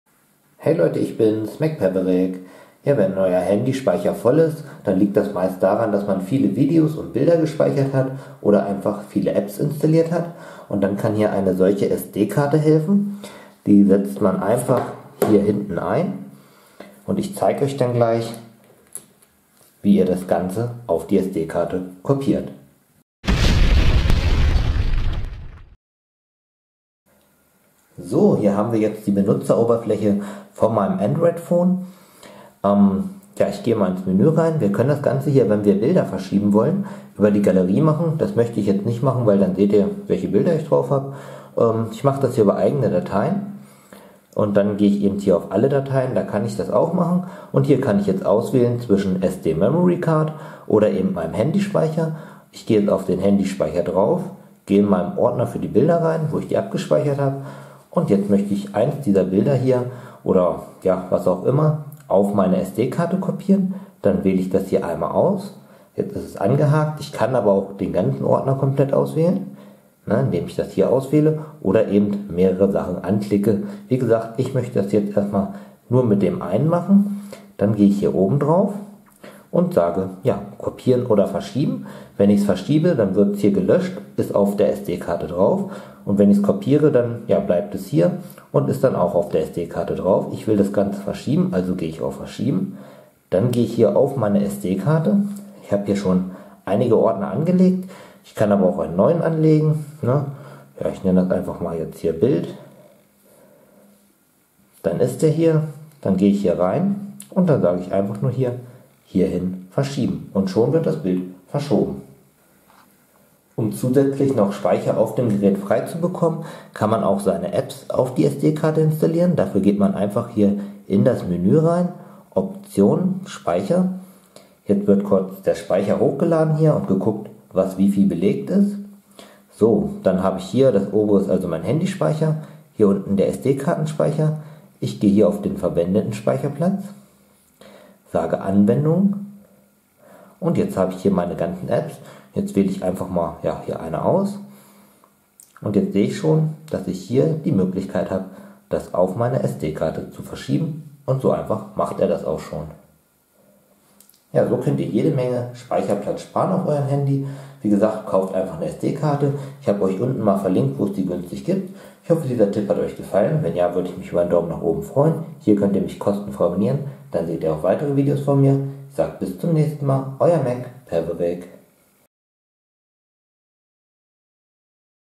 Bilder & Apps vom Handy auf Micro SD Karte verschieben – Smartphone Fotos Apps Daten kopieren
In diesem Video zeige ich, wie Ihr Bilder Apps oder andere Daten vom Handy auf die SD Karte verscheibt oder kopiert.

Günstige SD Karte kaufen